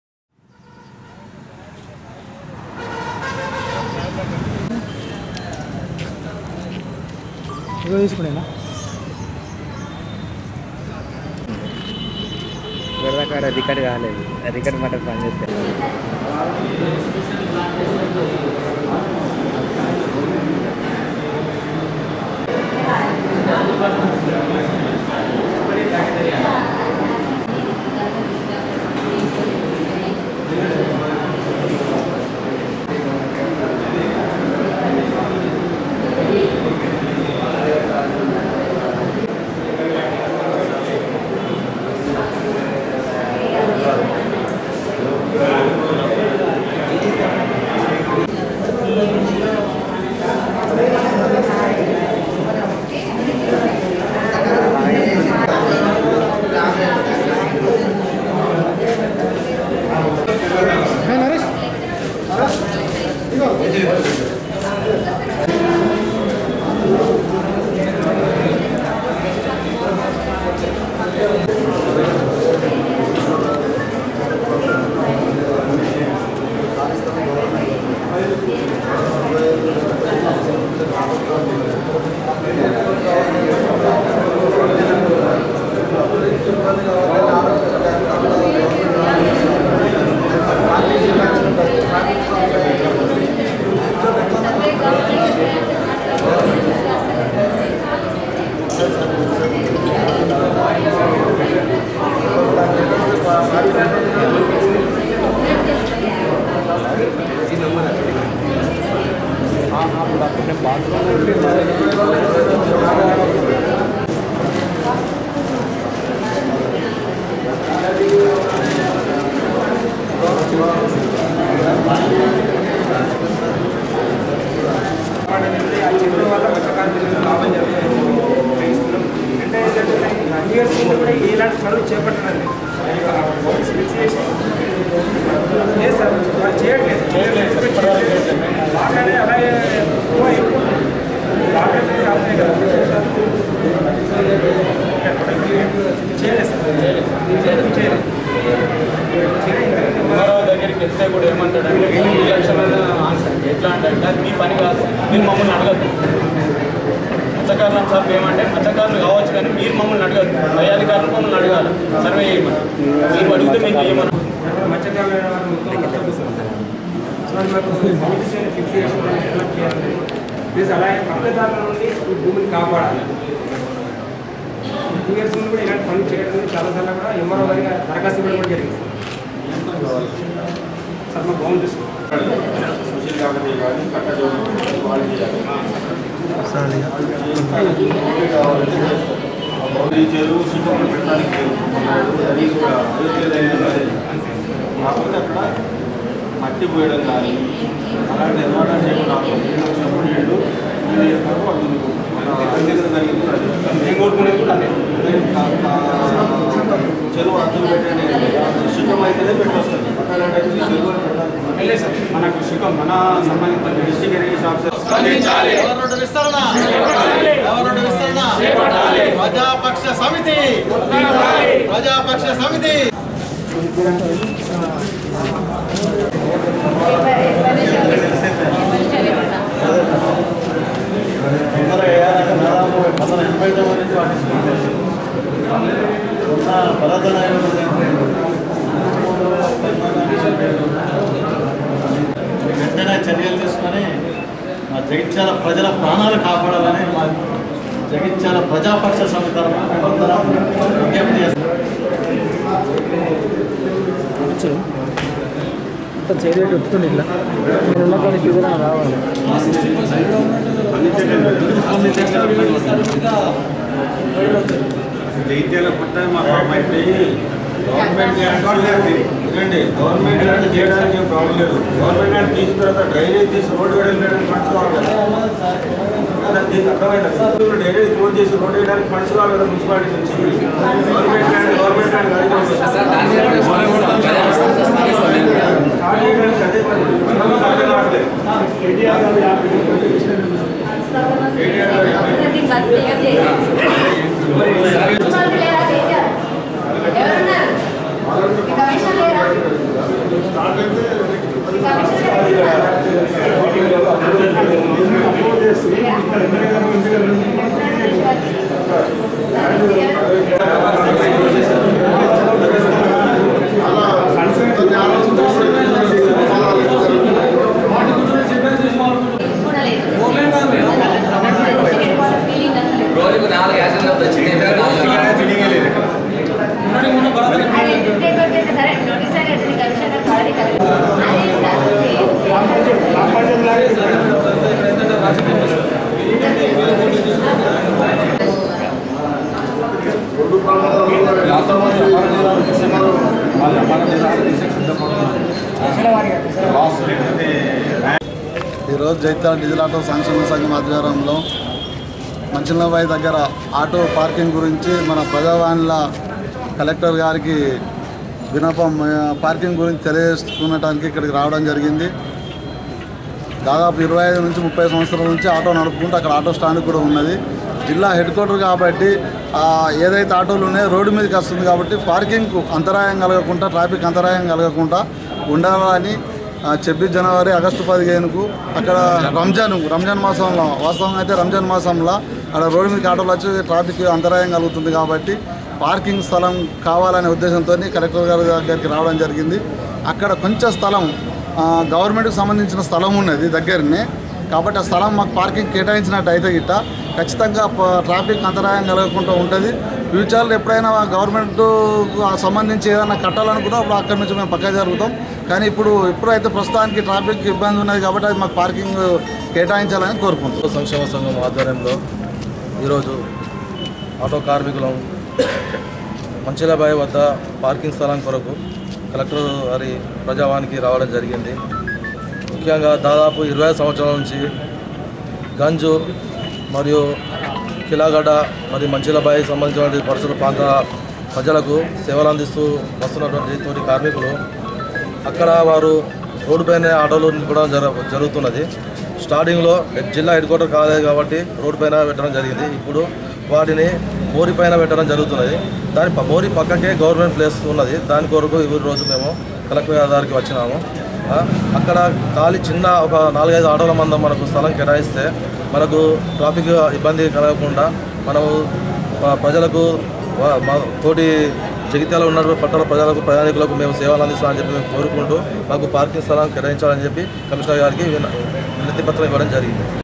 Sameera Channel News - Jagtial | 09.07.18
IMA భవన్ లో జరిగిన ప్రజావాణి కార్యక్రమం